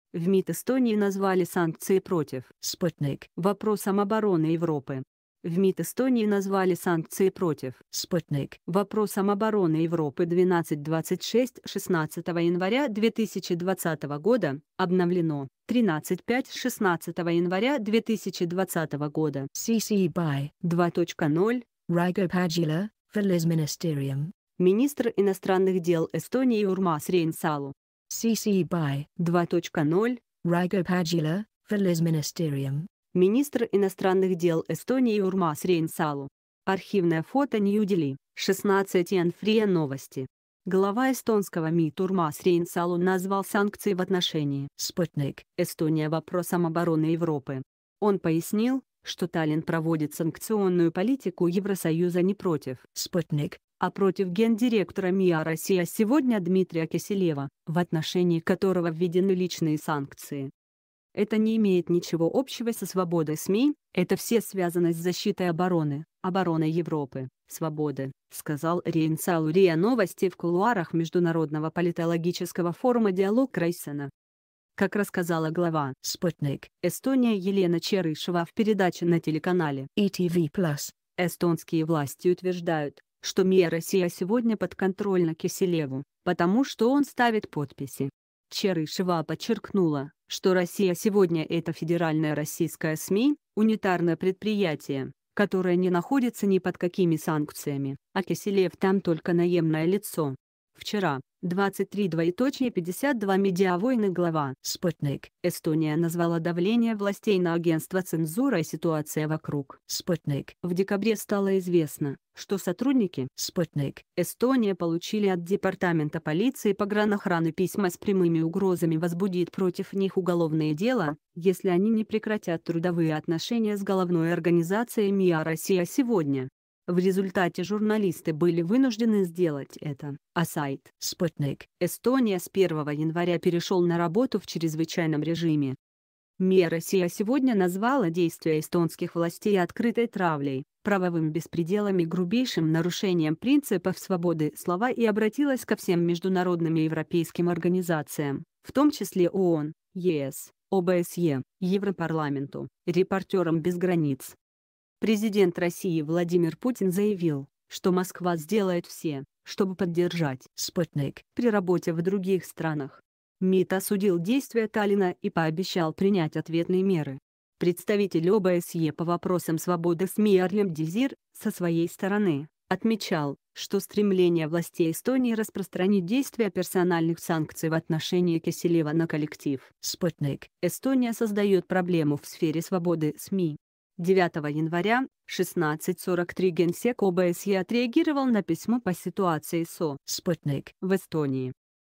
В МИД Эстонии назвали санкции против Sputnik вопросом обороны Европы
В МИД Эстонии назвали санкции против Спутник вопросом обороны Европы В МИД Эстонии назвали санкции против Спутник вопросом обороны Европы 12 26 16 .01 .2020 обновлено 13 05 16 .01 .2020 CC BY 2 .0 Raigo Pajula valisministeerium Министр иностранных дел Эстонии Урмас Рейнсалу CC BY 2 .0 Raigo Pajula valisministeerium Министр иностранных дел Эстонии Урмас Рейнсалу . Архивное фото НЬЮ-ДЕЛИ, 16 янв РИА Новости . Глава эстонского МИД Урмас Рейнсалу назвал санкции в отношении Спутник Эстони